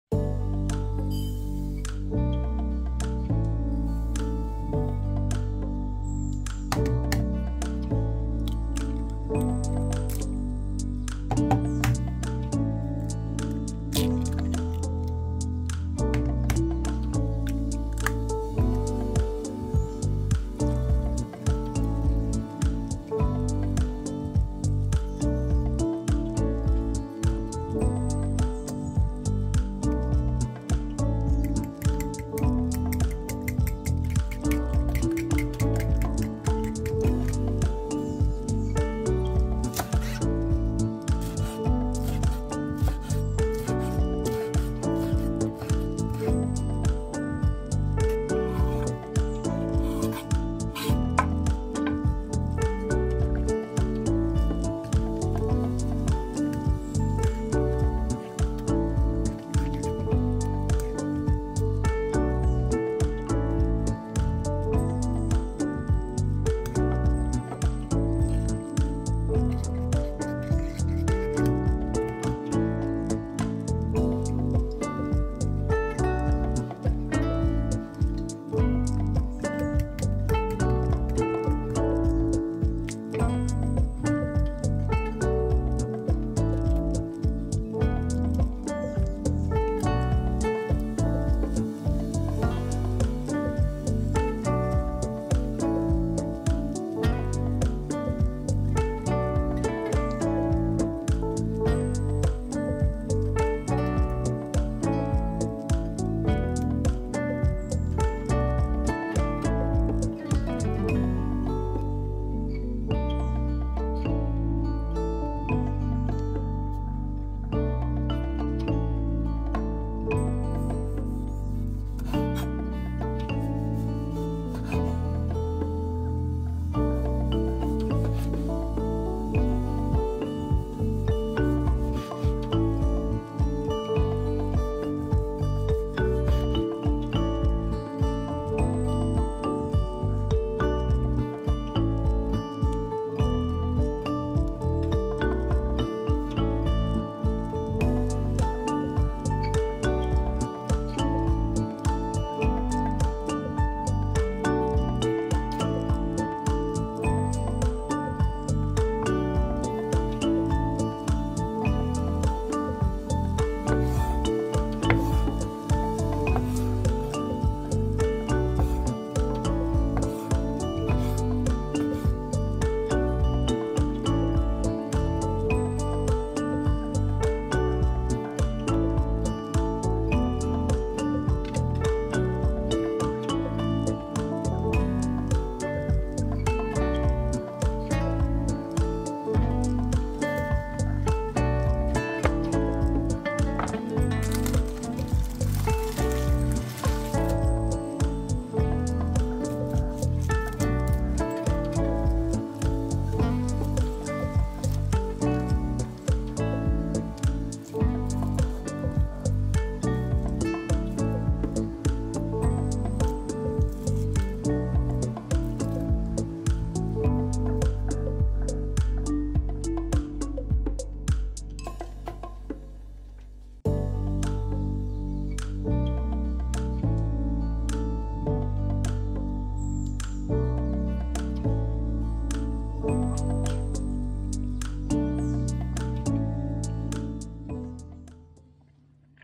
Dieses Ei mit Haferflocken Rezept ist so lecker, dass ich es fast jeden Tag kochen kann! Top rezept!
Guten Tag! In diesem Video zeige ich Ihnen, wie Sie mit Haferflocken, Eiern und Milch ein köstliches Frühstück oder Abendessen zubereiten! Sie werden den köstlichen Geschmack dieser Haferflocken-Ei-Pizza lieben! Haferflockenpizza ist lecker und gesund! Überraschen Sie Ihre Lieben mit Haferflockenpizza.
Bereiten Sie Ihren Lieben unbedingt ein solches Frühstück mit Eiern und Haferflocken zu, dann werden sie zufrieden sein!

2 Tassen Haferflocken
4 Eier
2 Tassen warme Milch
eine Prise Salz
umrühren und 5-10 Minuten ruhen lassen
Bund Petersilie
~ 2 Esslöffel Parmesan
ein paar Esslöffel Olivenöl
~ 4 Scheiben Käse
1-2 Tomaten
geräucherte Wurst
eine Handvoll Oliven
~50-70 g Mozzarella
ca. 20–25 Minuten bei 200 °C backen
in Portionen schneiden